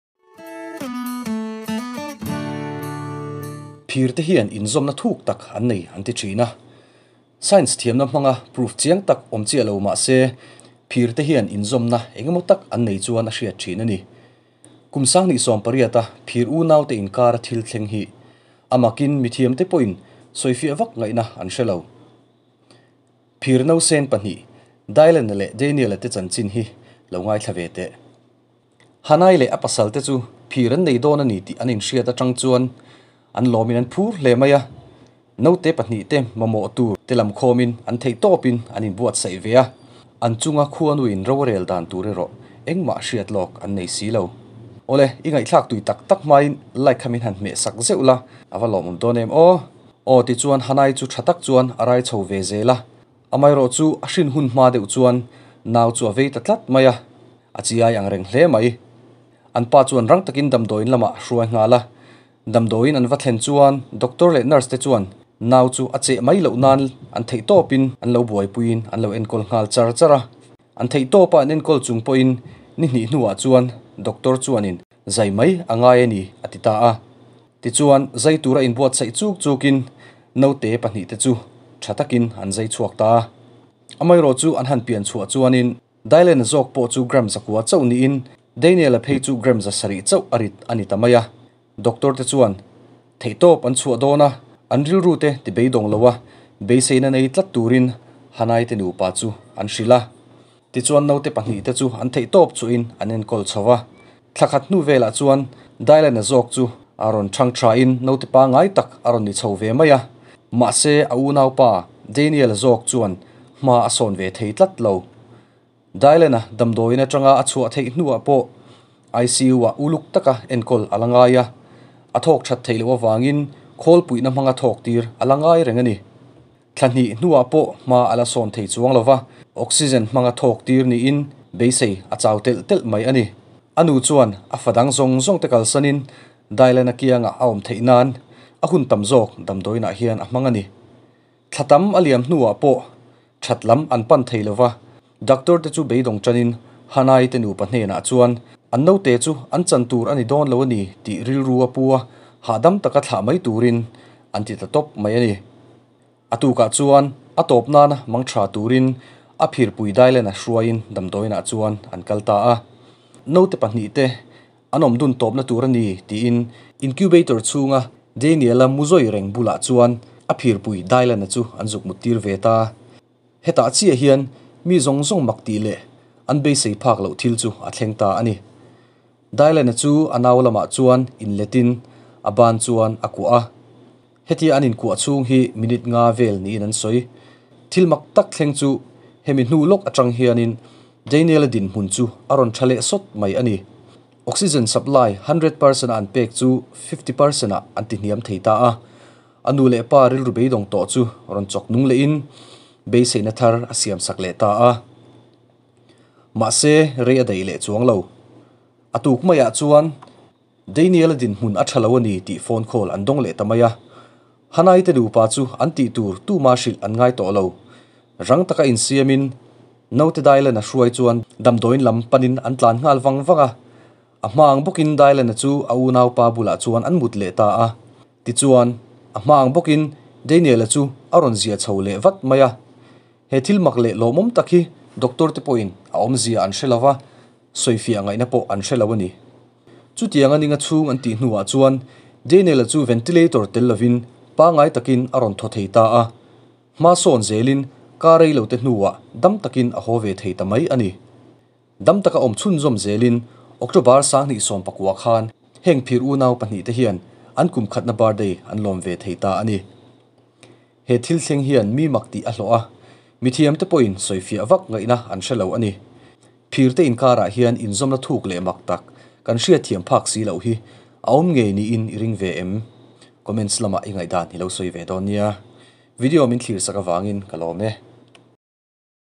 A phir pui damlo bulah an muttira... minute 5 hnuah thil mak a thleng...!!
Naute thi mai tawh tura an ngaih bulah a phirpui an muttir a thil mak a thleng...